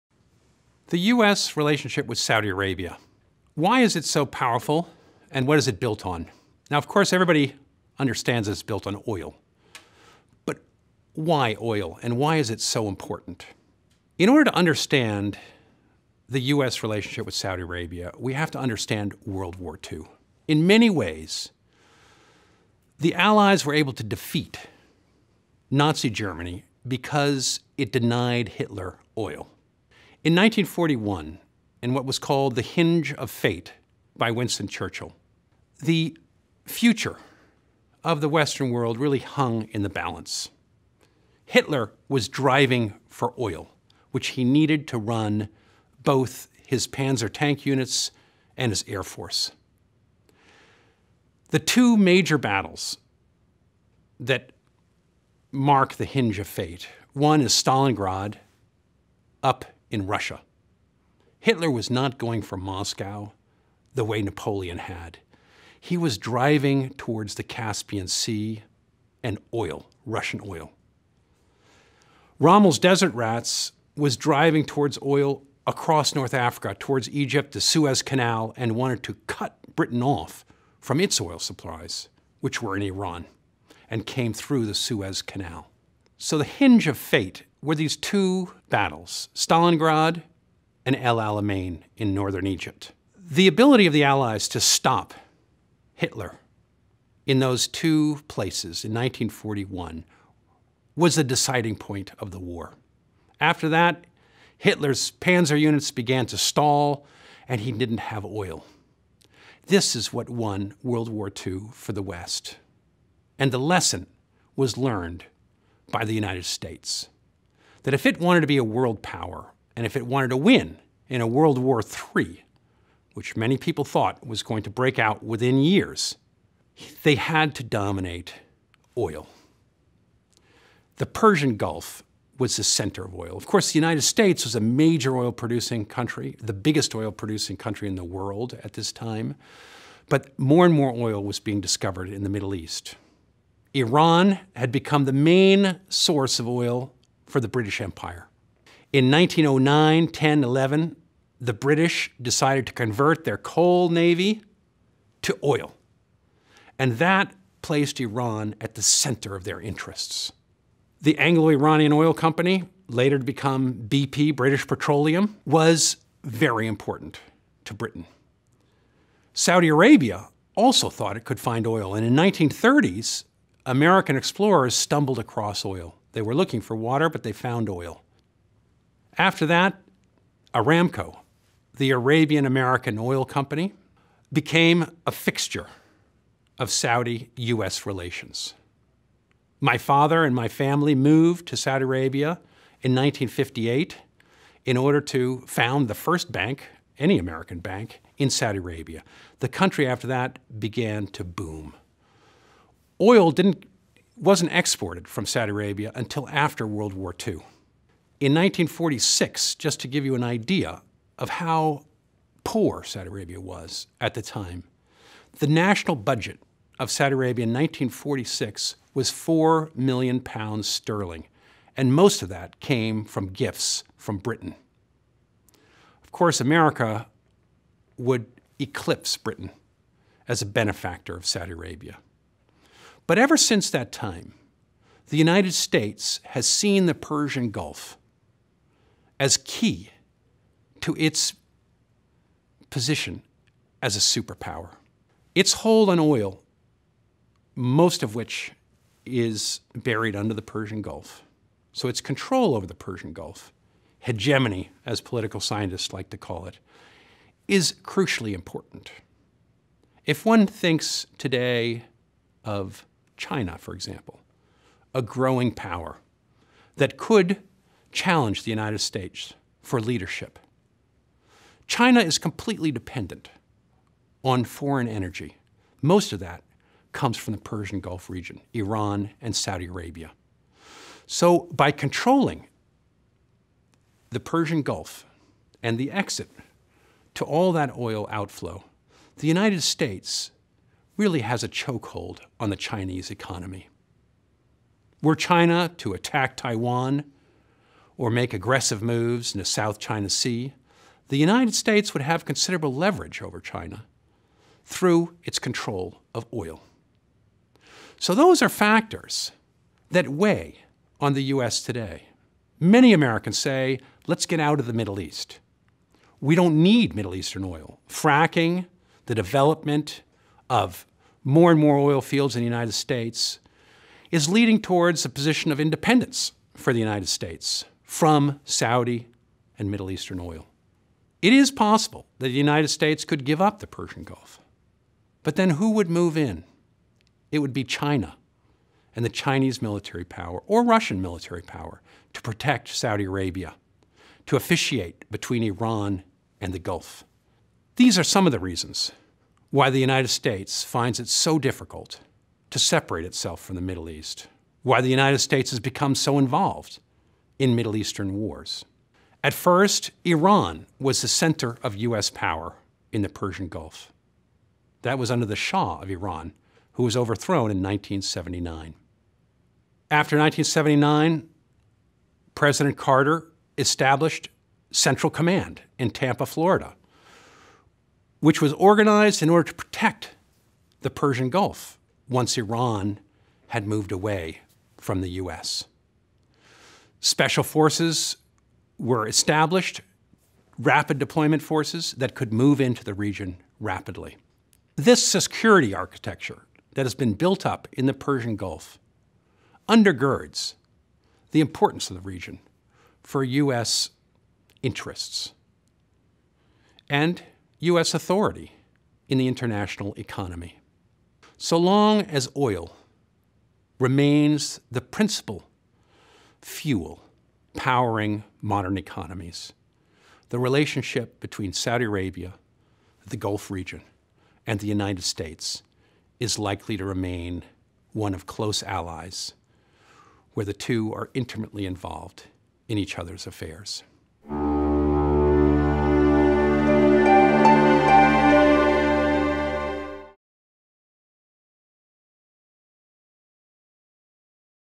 The US and Saudi Arabia
“International Relations in the Middle East“ is an online course on Janux.

Joshua Landis Ph.D. Associate Professor and Director of the Center for Middle East Studies in the College of International Studies